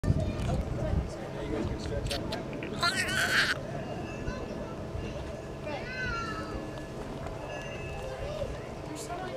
Meerkats Playing
A Day with the Awesome Animals at the Zoo.!
Cheyenne Mountain Zoo - Colorado Springs, CO

Also Visit the Garden of the Gods and Seven Falls while your there.
Share these videos of Great Animals at the zoo with your friends and kids.